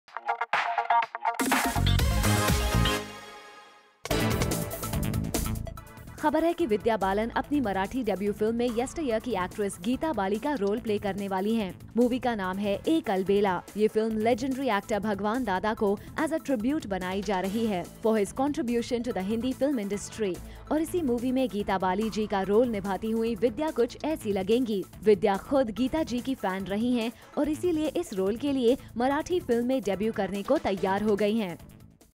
CHECK OUT!! Vidya Balan's FIRST LOOK From Her Marathi Film | Bollywood News
Vidya Balan's first look as Geeta Bali in Marathi movie 'Ek Albela'.